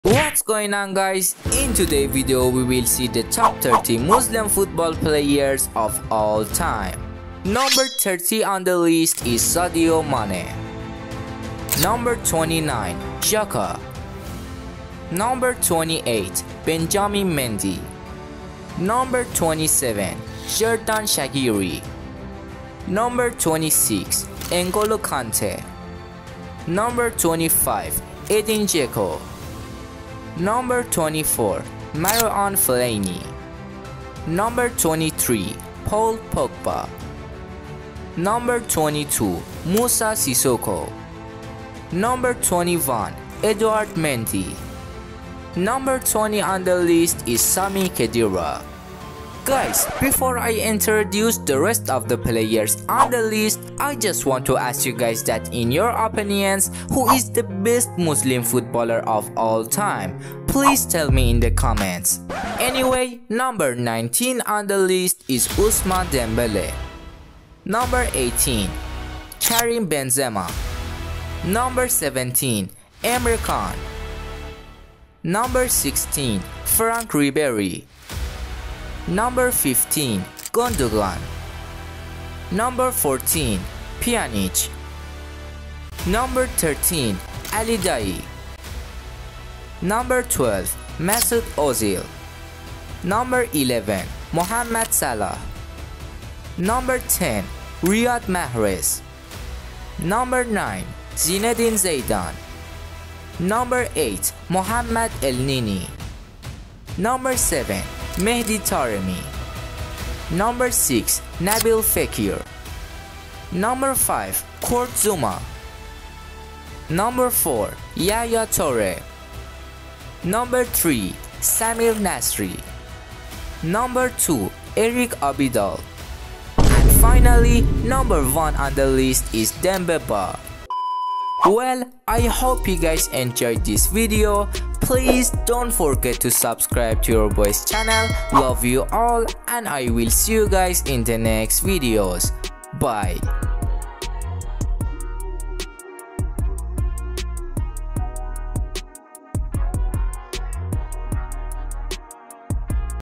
Best Muslim Football Players
In today video guys we will see best muslim football players in the world
( top 30 muslim football players)

please don't forget to support your boy's channel🥺

I use videos less than 5 seconds and also I remove original voice of videos
I write every channels name too